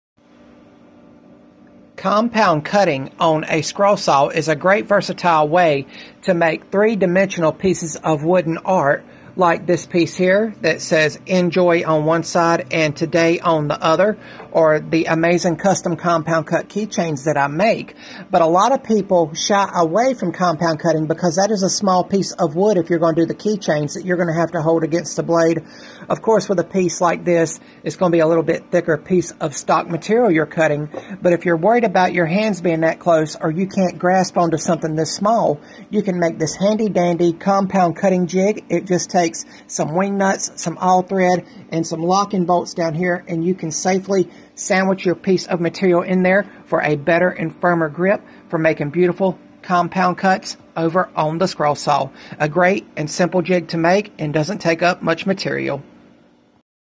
Simple Jig For Scroll Saw Compound Cutting
Just a quick short, showing an amazing little jig that you can easily make to help you get a better grip on smaller pieces for scroll saw compound cutting

Remember, If I Can Make It Or Do It So Can You !
God Bless !

Mailing Address;

***NEW Instagram Account!***